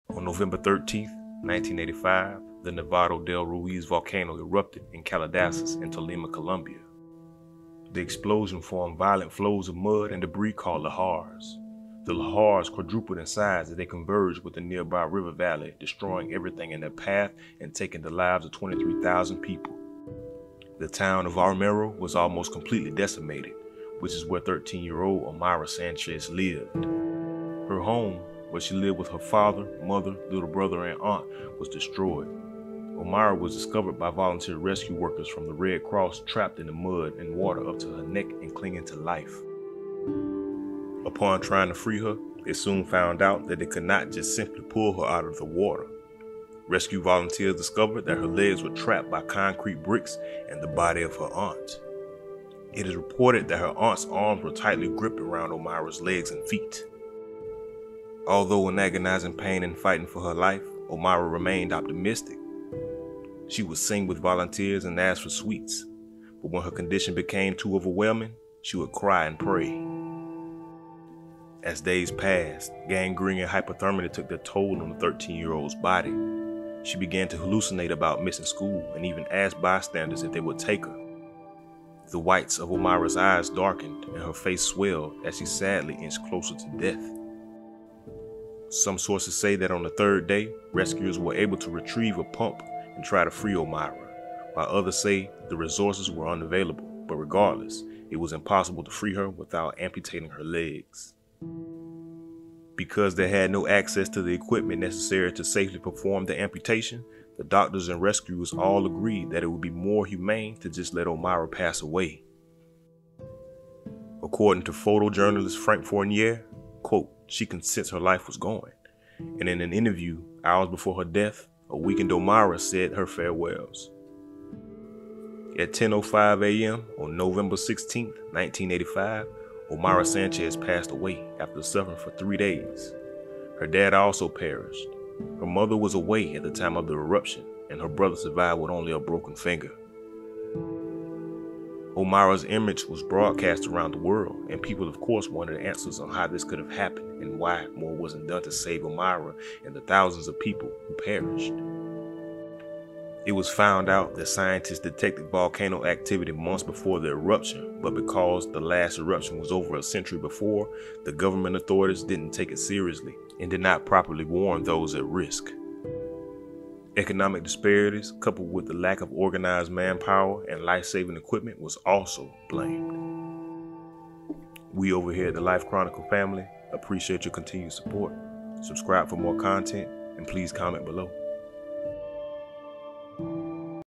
They Couldn't Save Her | Omayra Sanchez
Omayra Sanchez was a young girl whose life sadly ended. Her image has circulated around the world, but few know her story.